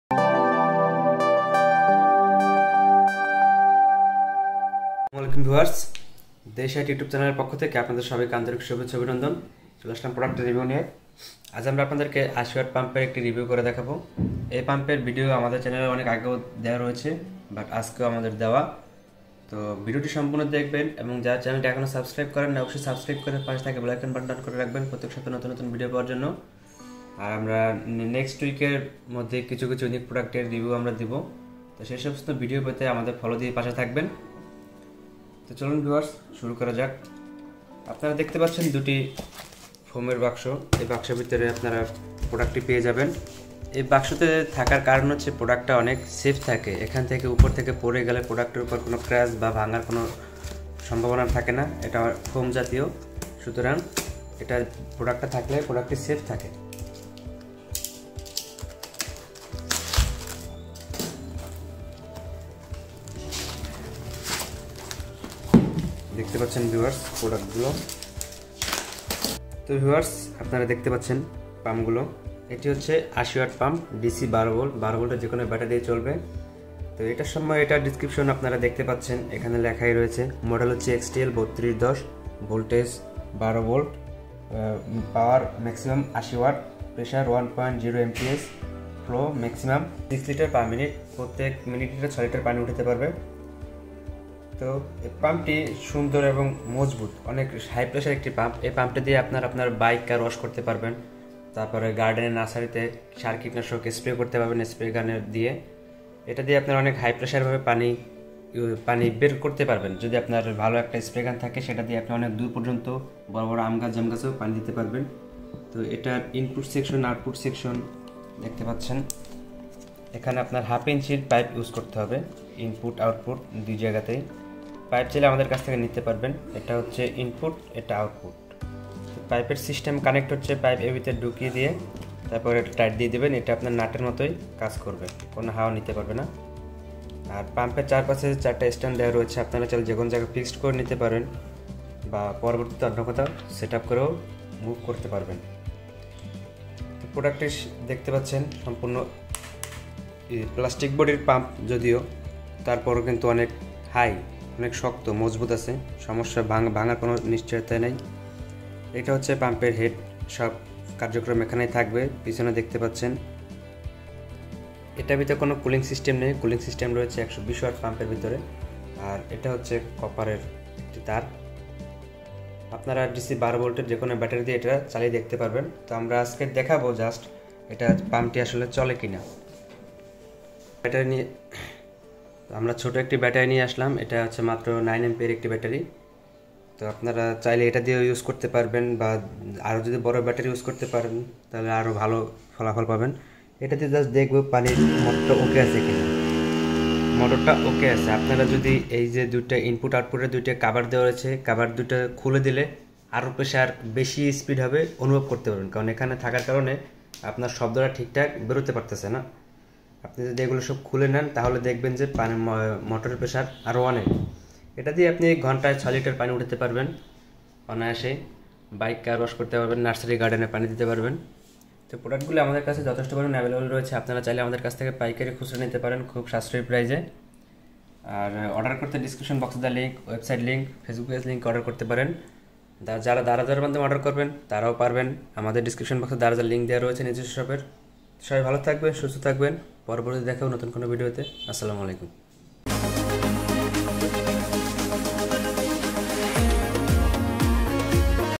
12V 80W High Pressure Water Pump | XTL 3210 Pro Pump | আম, লিচু বাগানে স্প্রে পাম্প
Pro 12 Volt 80 Watt 100Psi 6L/Min High Pressure Water Pump | আম লিচু বাগানে স্প্রে পাম্প

12 Volt 80 Watt High Pressure Water Pump | প্রতি ঘণ্টায় তুলতে পারবেন ৬ লিটার পানি | এই পাম্প দিয়ে গাড়ি ওয়াস করতে পারবেন, আম, লিচু, বাগান বা নার্সারিতে পানি দিতে পারবেন তাছাড়া কীটনাশক স্প্রে পাম্প করতে পারবেন।

অর্ডার করতে ফোন করুনঃ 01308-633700, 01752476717 নাম্বারে
আমাদের ওয়েবসাইটে অর্ডার করতে ভিজিট করুনঃ

দারাজে আমাদের পন্যের ক্রয় করতে ভিজিট করুনঃ